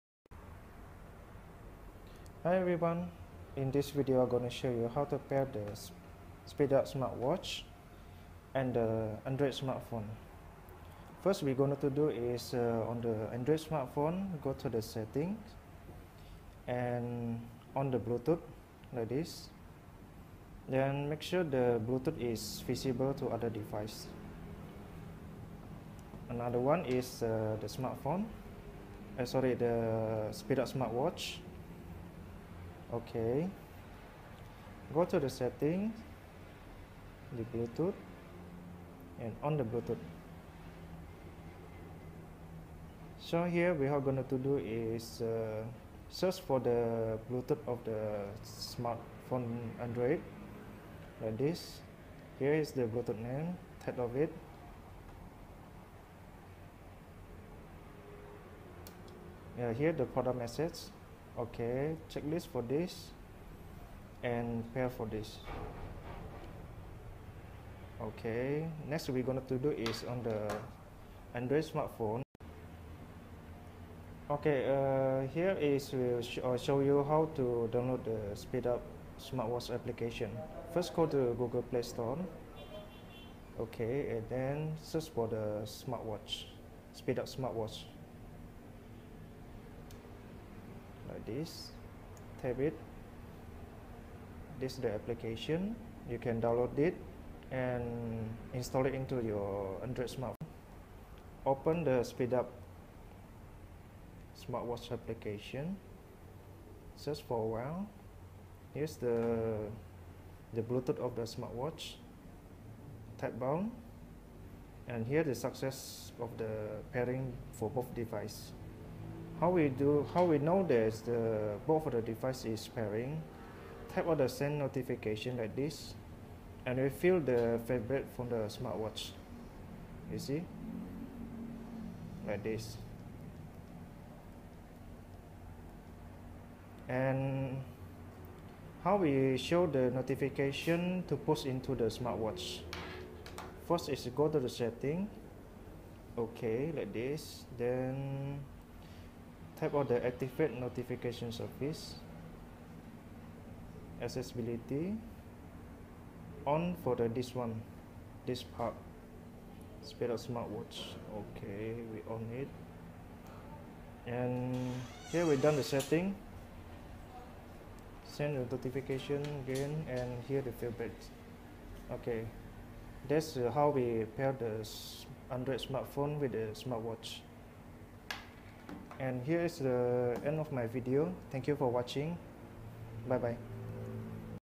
How to paired your SpeedUp SmartWatch with Smartphone
This video will give you direction to paired your SpeedUp SmartWatch with your Smartphone.
PS: you need to download "SpeedUp SmartWatch" application from Google Play Store.

Pin BBM: 2A4DB3B8